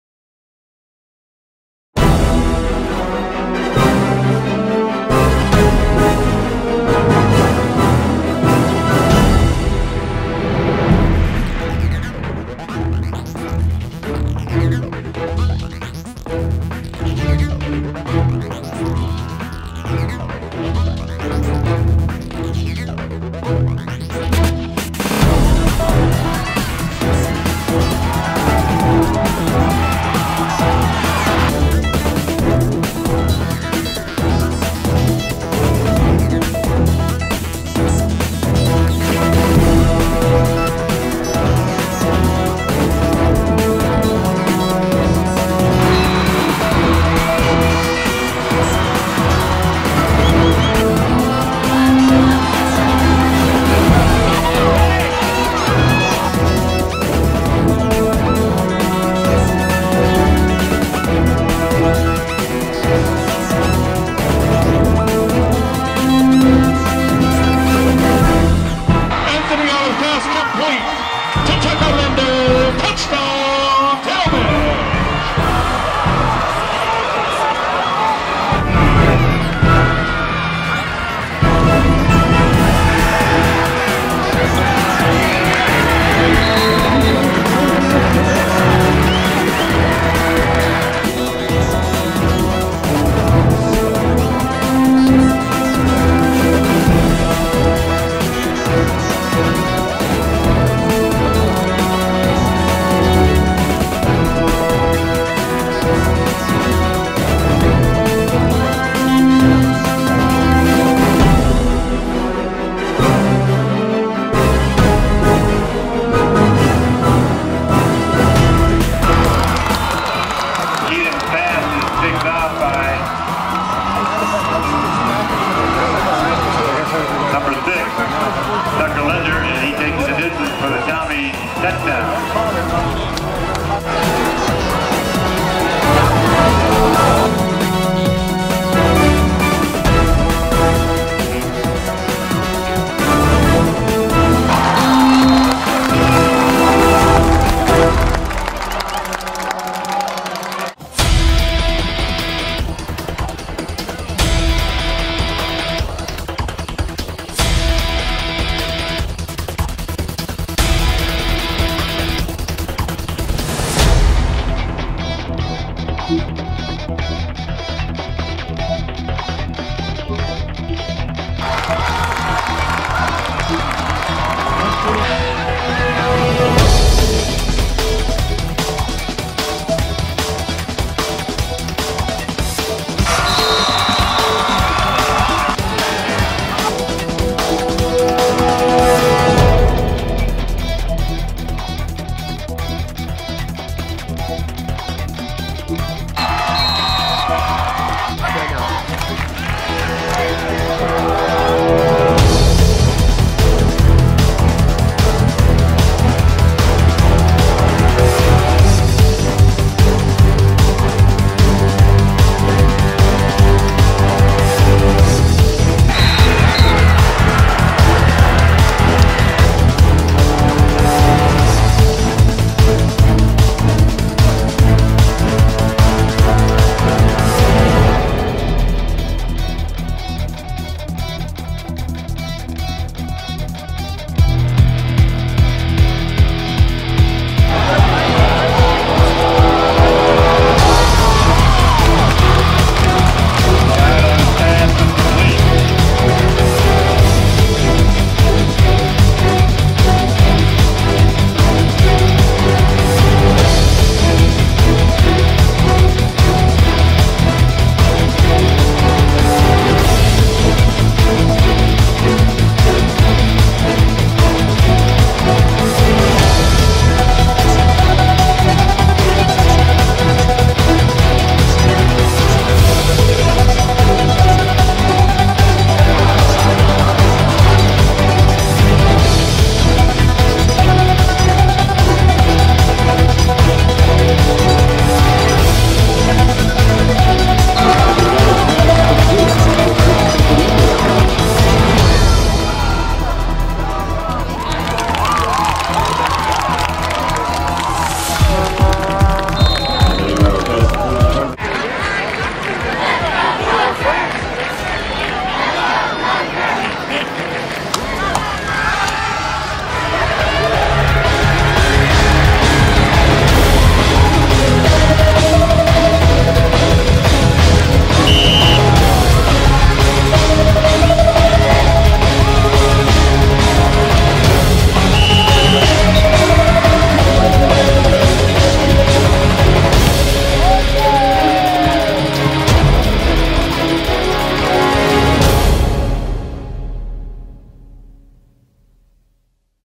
Tucker Linder FB Highlights 3 GMS 2011
Senior Tucker Linder football highlights through the first three games of 2011.